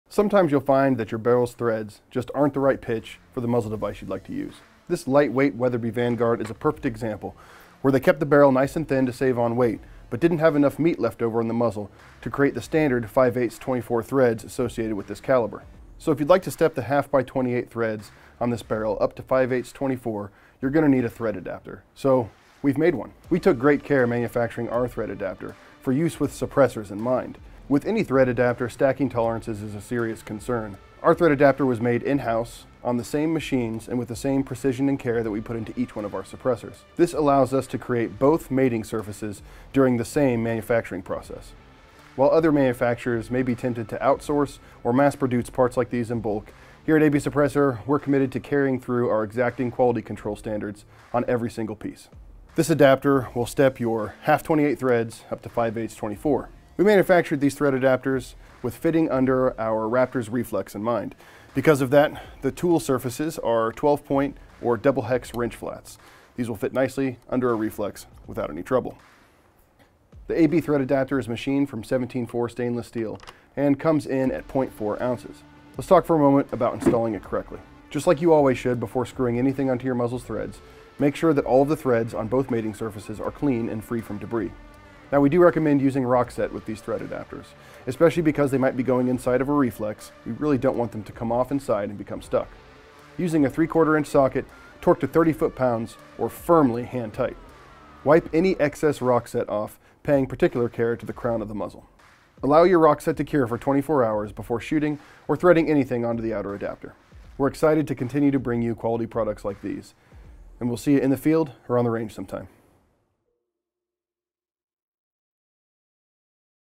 Thread Adapter for Suppressors?
Sometimes there just isn't a perfect solution to mate your suppressor adapter to threads on the muzzle of your rifle. Whether it needs to fit under a suppressor reflex like on the AB Raptor, or you need a thread adapter to avoid purchasing another flush mount from, the mating surfaces must be perfect. Concentricity cannot be compromised.

Most manufactures may be tempted to outsource bulk muzzle bushings to the lowest bidder made with techniques unknown. Made in-house using the same precision AB Suppressor uses to make our best in class reflex suppressors, the AB Thread Adapter offers the ideal solution for a 1/2x28 to 5/8x24 adapter.